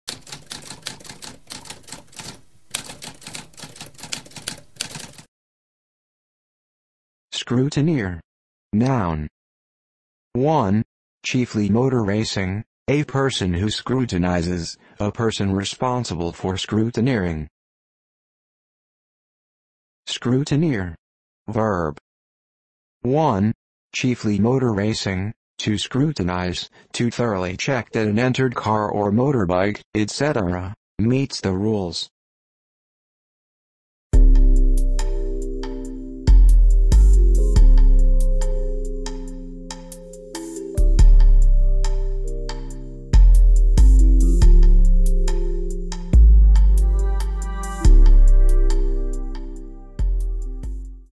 What does scrutineer mean?
A spoken definition of scrutineer.

Intro Sound:
Typewriter - Tamskp